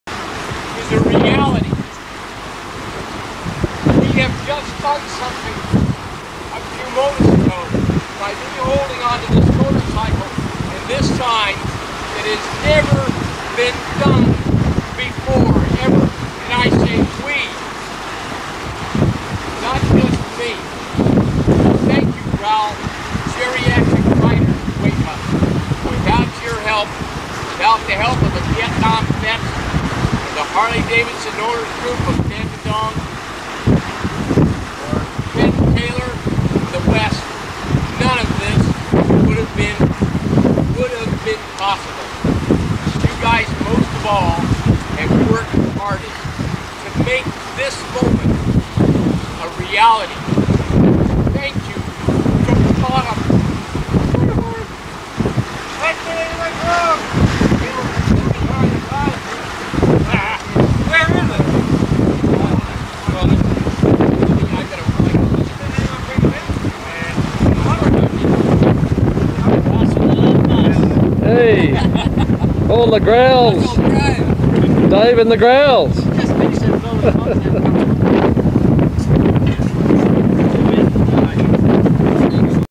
CAPE YORK HARLEY DAVIDSON only one to ever make it (the tip)
This is the first and only harley thats made cape york in AUSTRALIA.
im going to be the second but on a ultra classic.